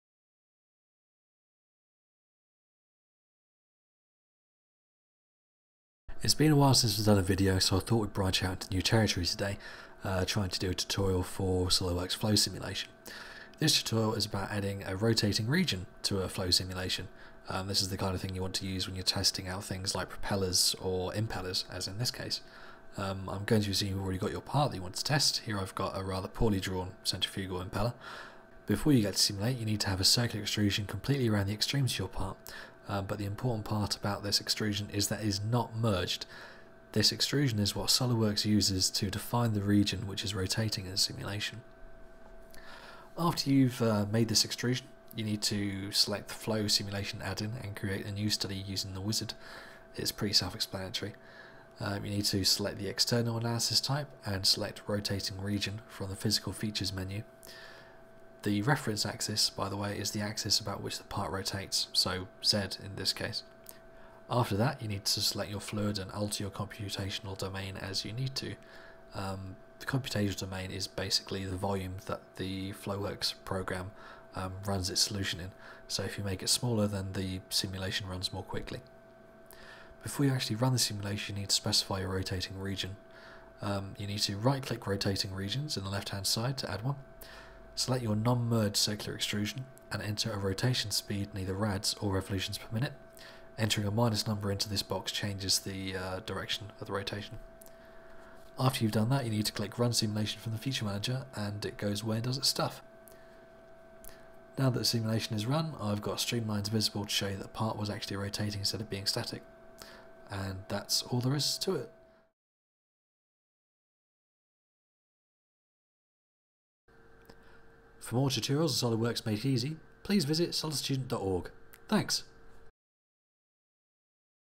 Solidworks Tutorial - Adding a rotating region to a flow simulation
A quick tutorial on how to add a rotating region to a flow simulation in Solidworks - useful for testing things that spin, so things like propellers, rotors, impellers and turbines.

If you have any questions, please ask me in the comments and I'll do my best to get back to you. :)